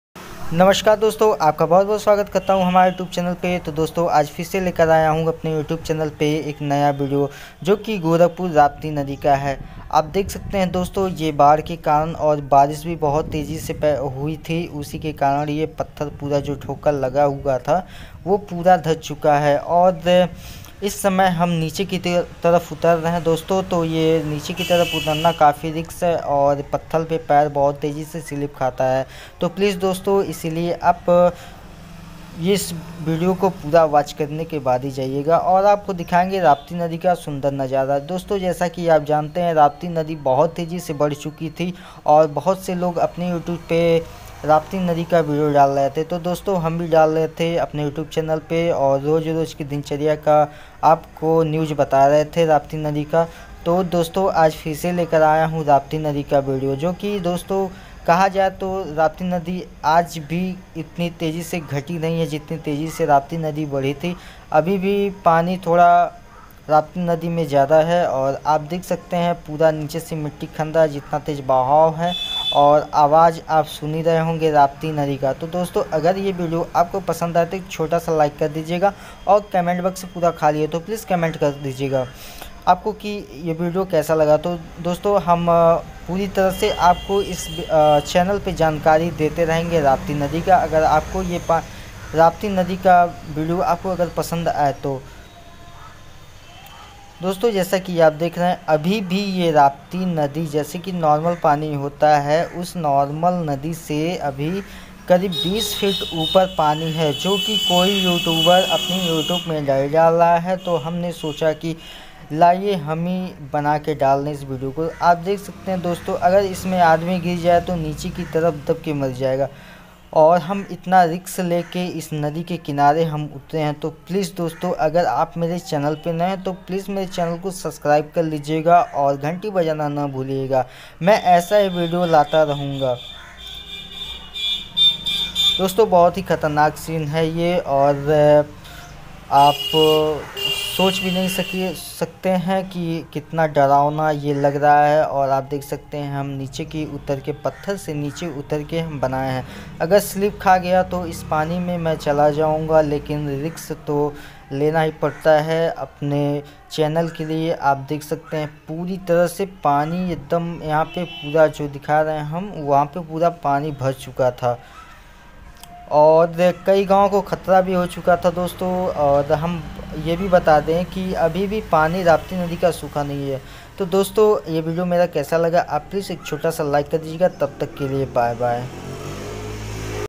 गोरखपुर राप्ती नदी का जलस्तर | आज भी नदी का जलस्तर बढ़ा हुआ है Gorakhpur city..
aaj bhi itni Raat ki Nadi badi hui hai